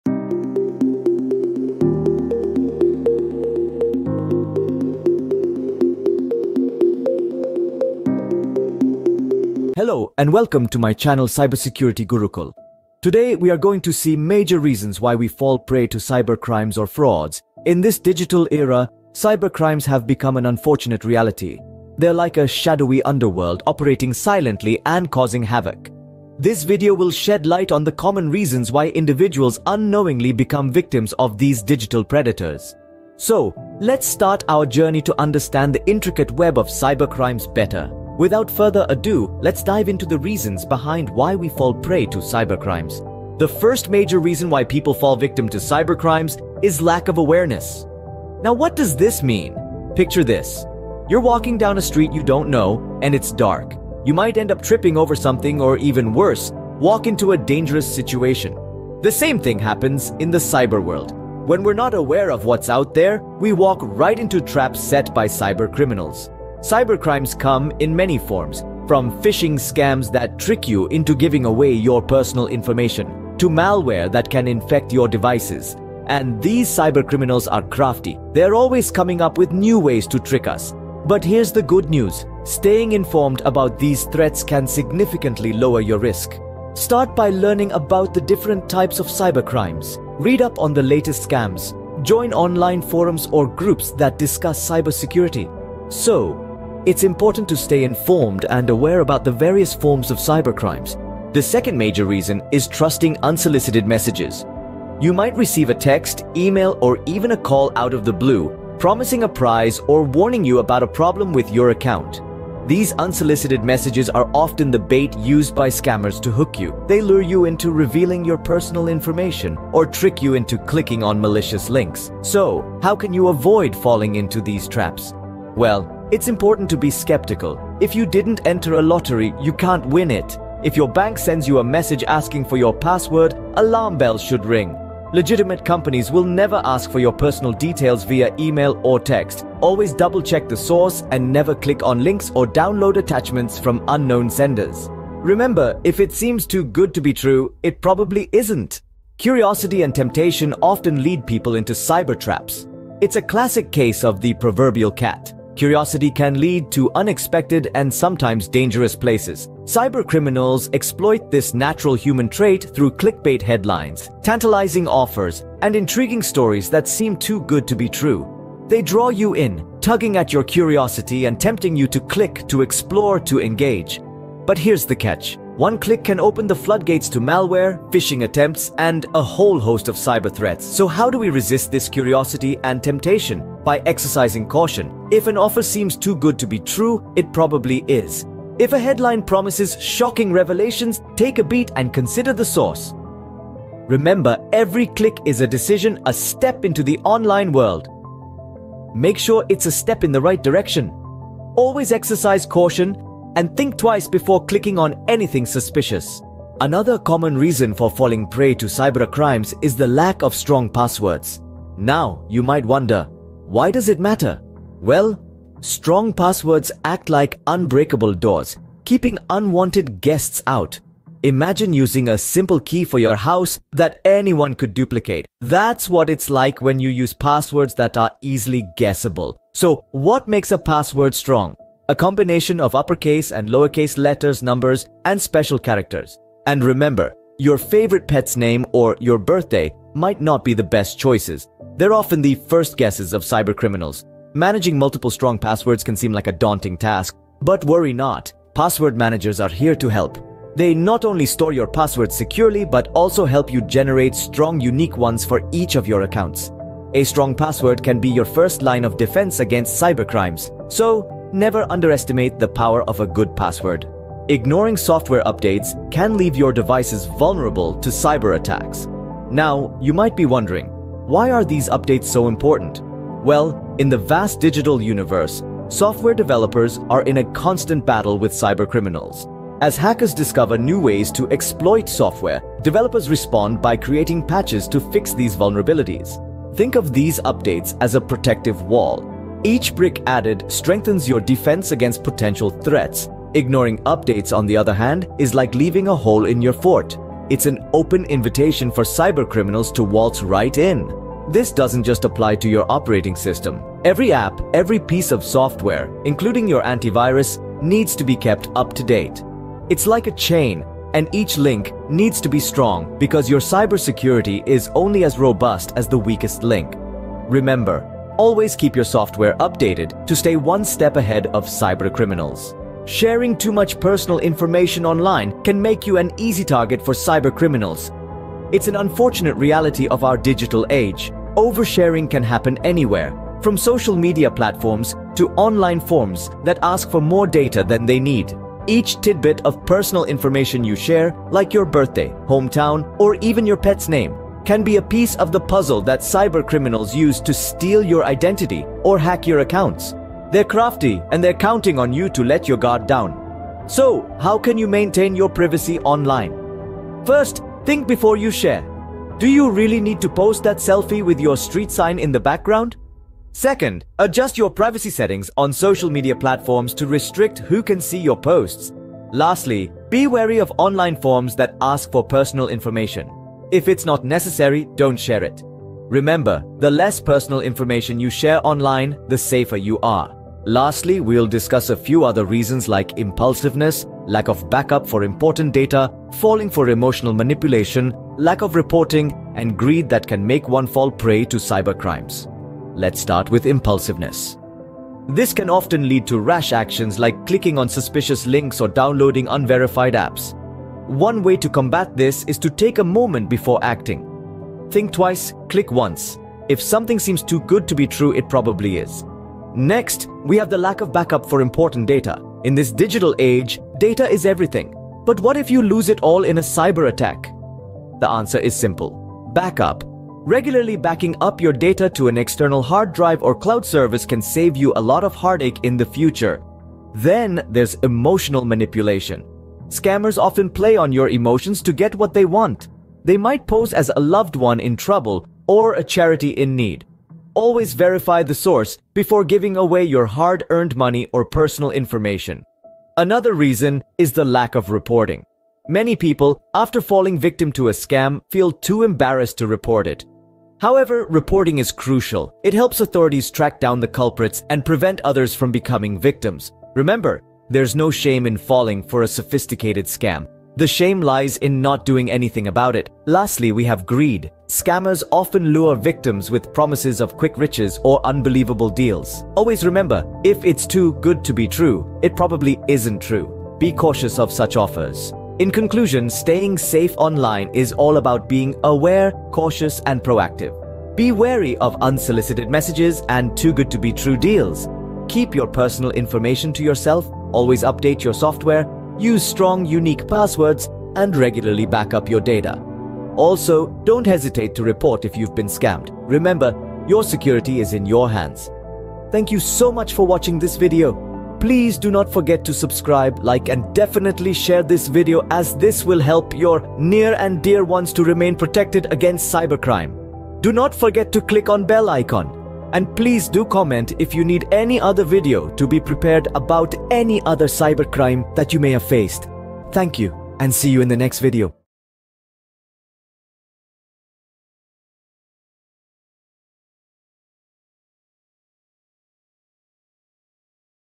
Why we become victim of online scams
Knowledge is your shield, Vigilance is your Sword.

In this eye-opening exploration, we delve into the underlying reasons that lead common people to become unsuspecting victims of cyber traps and scams. Understanding these vulnerabilities is the first step towards building a robust defense.

The digital age has brought immense convenience and opportunities, but it has also exposed us to new vulnerabilities. In this video, we dissect the psychology and circumstances that make individuals susceptible to cyber crimes. Whether it's the allure of online offers, the urgency created by social engineering, or simply a lack of awareness, we uncover the factors that can put you at risk.

But this video is not just about highlighting weaknesses; it's also about empowerment. We provide essential tips and guidance on how to remain vigilant against these vulnerabilities. By the end, you'll have a clearer understanding of the caution to exercise while navigating the cyber world, and you'll be better prepared to protect yourself and your digital assets.

Join us in this insightful journey towards bolstering your cyber awareness and fortifying your digital defenses. Knowledge is your armor against cyber threats.